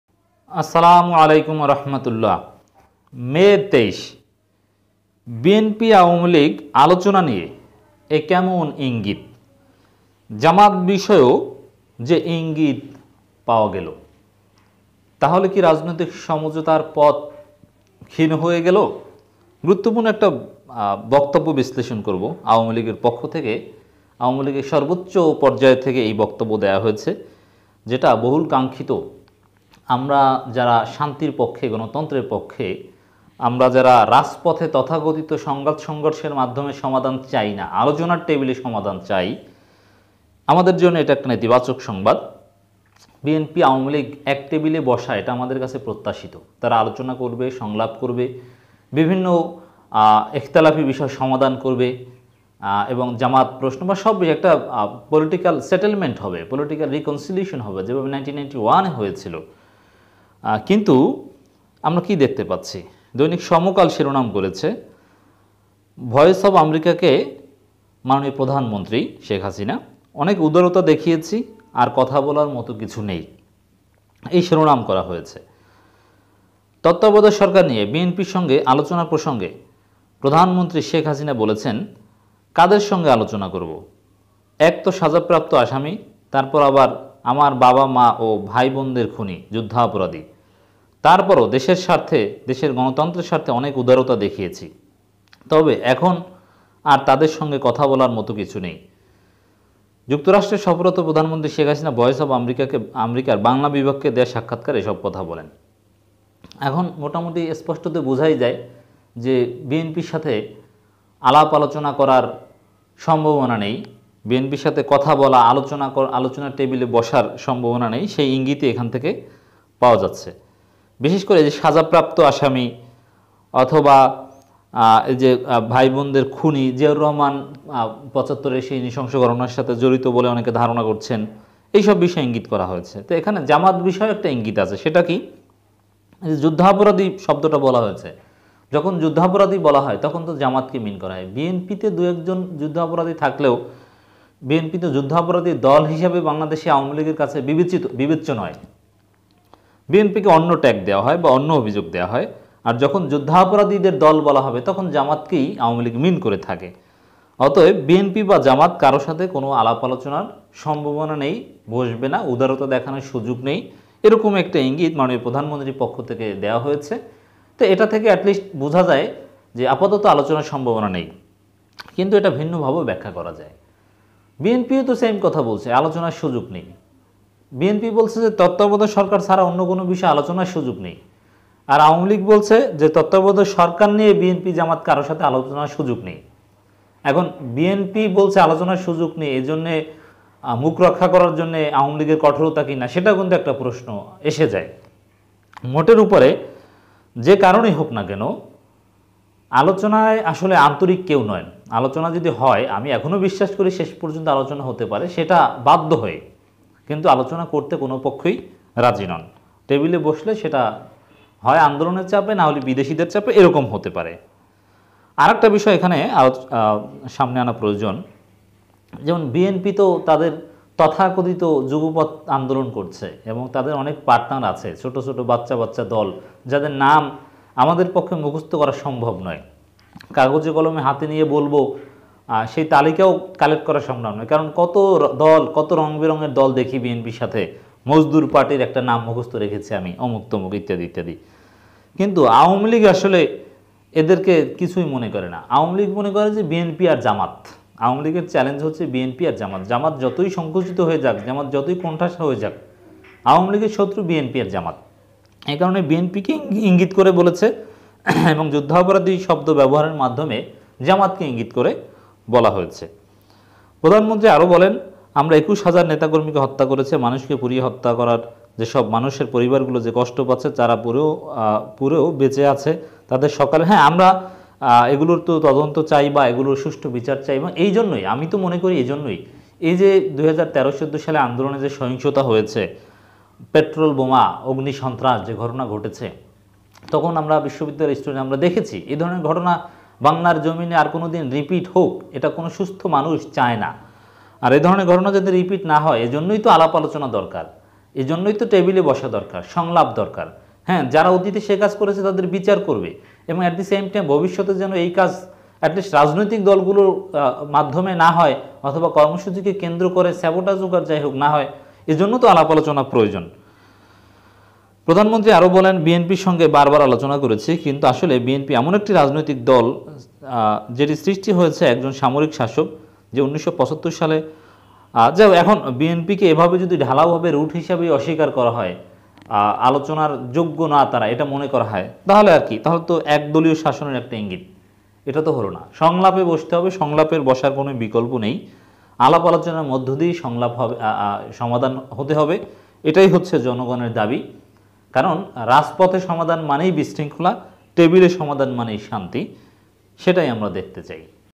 মে'২৩- বিএনপি- আওয়ামীলীগ আলোচনা নিয়ে এ কেমন ইংগিত||জামায়াত বিষয়েও যে ইংগিত পাওয়া গেলো
তাহলে কী রাজনৈতিক সমঝোতার পথ ক্ষীণ হয়ে গেলো।